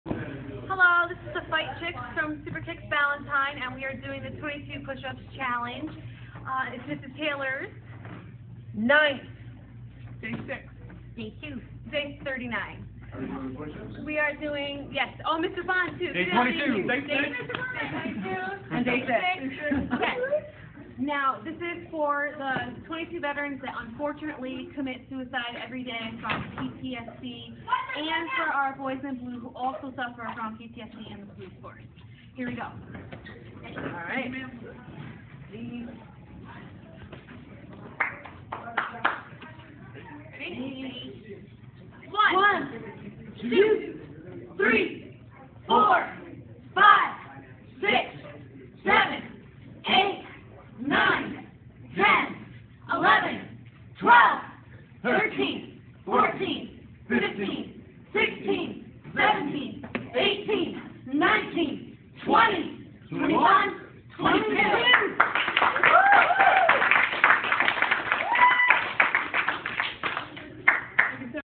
22 Pushups Challenge
I have accepted this challenge to honor those who serve in our Military.  I am pushing to raise awareness for veteran suicide prevention through education and empowerment.